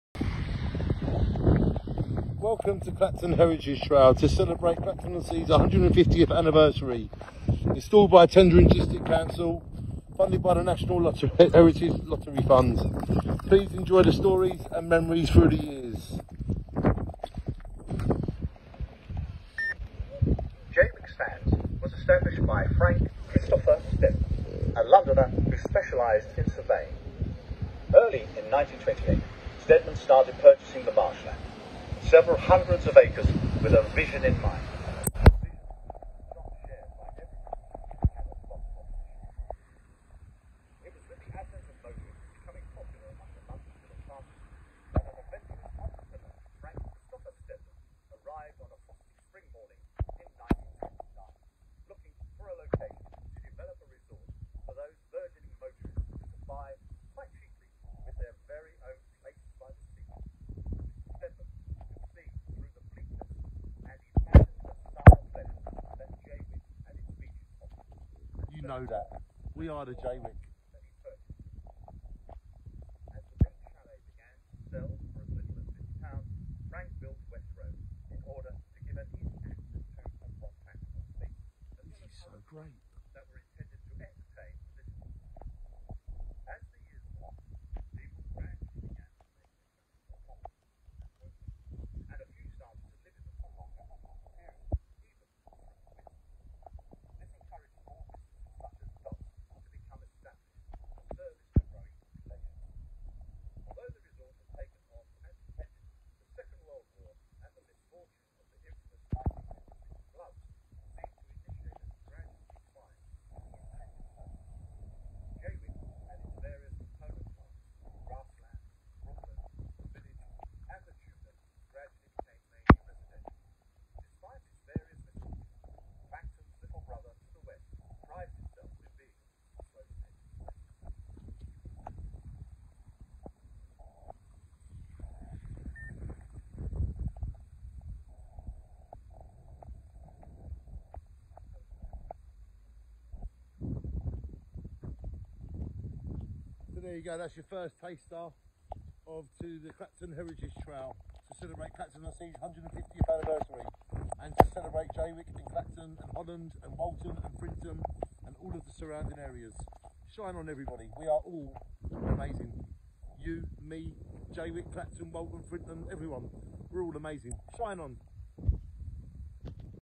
I press play on Clacton heritage trial number one in Jaywick talking about our history and our minia
I press play on Clacton heritage trial number one in Jaywick talking about our history and our miniature railway.